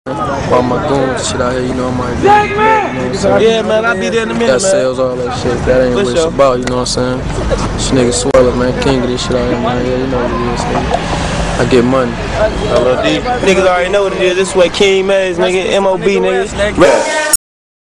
Rest in Peace Swella
Hanging on 8th street for 17 seconds with Swella.Rest in Peace to Demarco Green